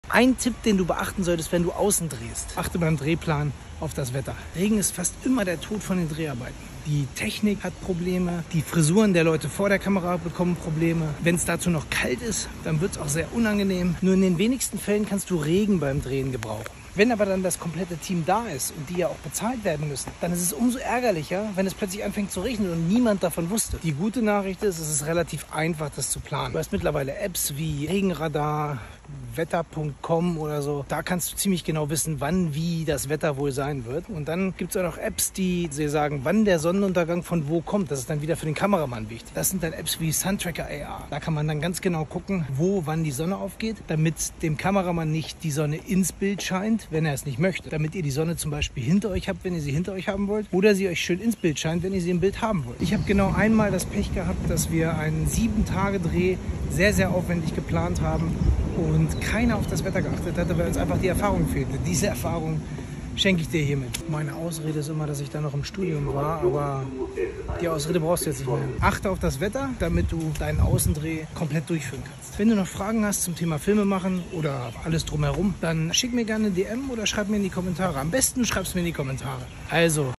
FILMSCHOOL: Achte immer auf das Dreh-Wetter
Regen kann ein Problem bei Dreharbeiten sein, aber auch Hitze, starker Wind oder zu viel Sonne.

Check das Wetter vor den Dreh: Wetterapps helfen, wenn du wetterbedingte Probleme frühzeitig erkennen willst.

Plane Innenaufnahmen mit ein um auszuweichen, falls das Wetter nicht mitspielt, um den Dreh fortzusetzen.

- Regenschutz für Ausrüstung ist wichtig.
- Achte auf die Sicherheit aller Beteiligten. Die Gesundheit und Sicherheit deiner Leute geht immer vor.